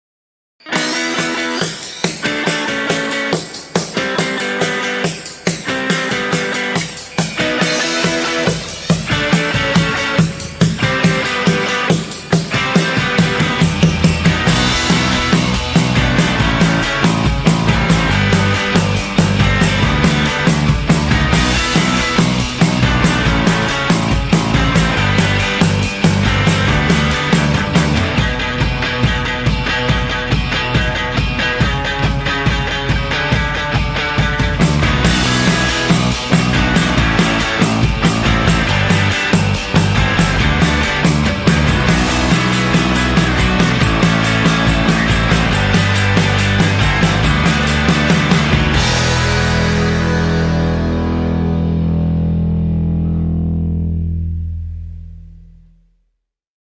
Introducing American Class A – Softube
✮ Dream Big, Sound Bigger
The American Class A console emulation from Softube is here. Available as a Console 1 channelstrip or as a standalone plug-in, it brings all the power and punch of the classic American console sound. ✮